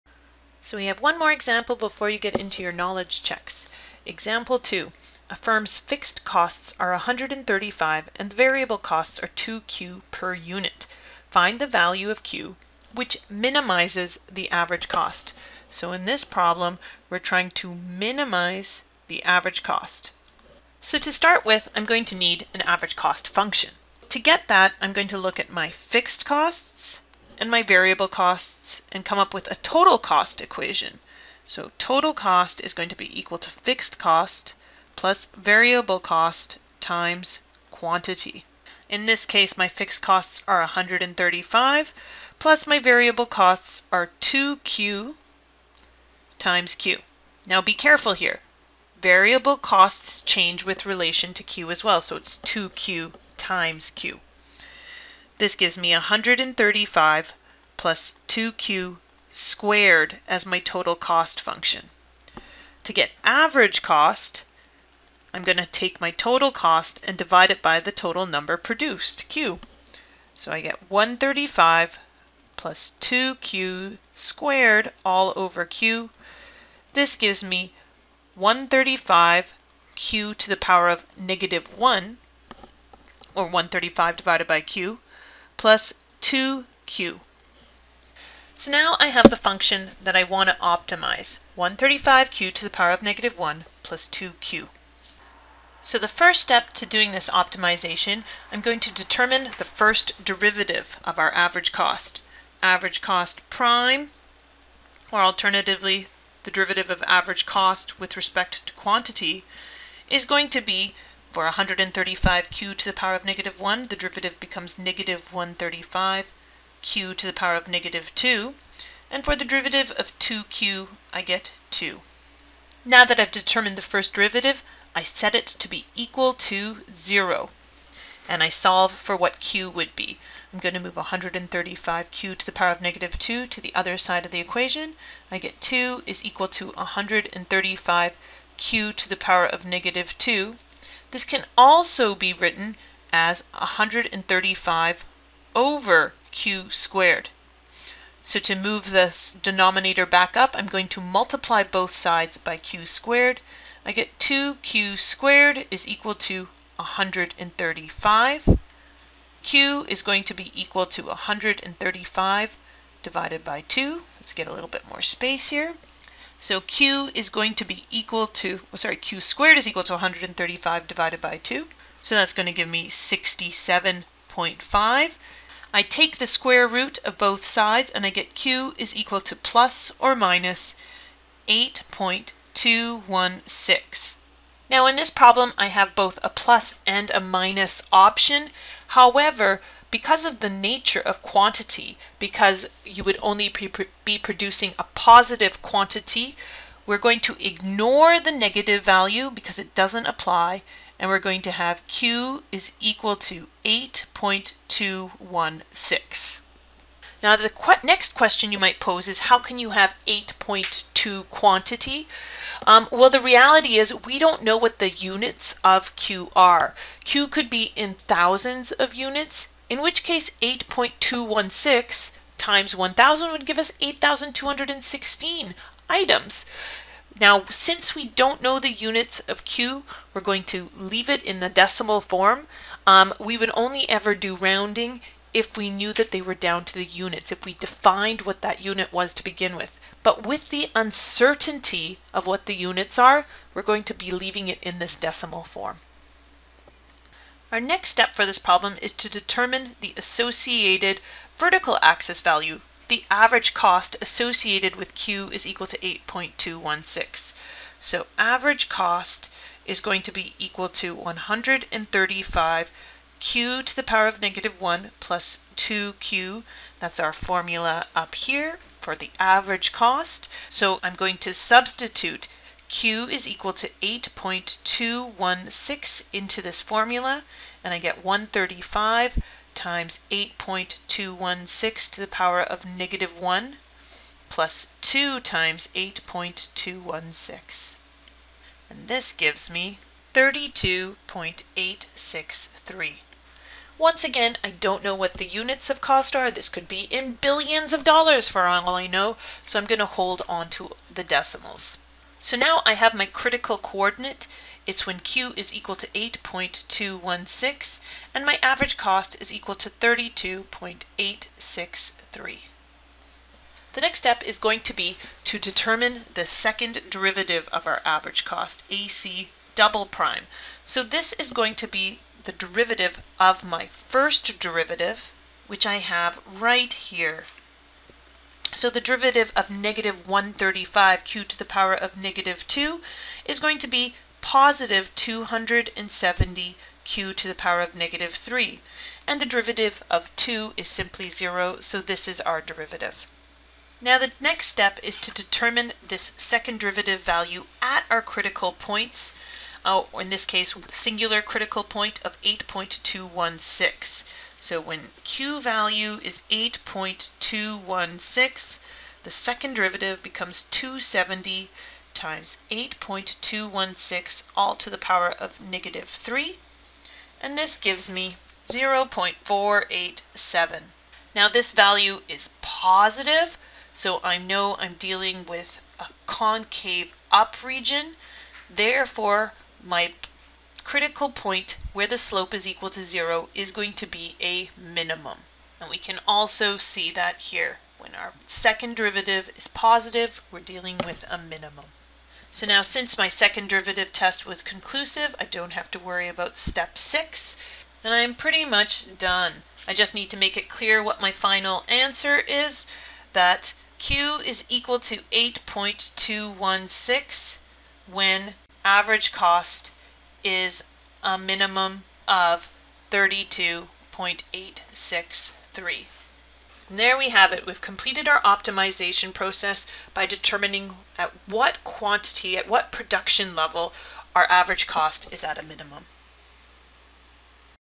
4.03C Optimization Example 2
This video provides an example of the optimization of an average cost function. We first determine the average cost function. The production level (Q) at which average cost is a minimum is then determined, along with the corresponding minimum average cost.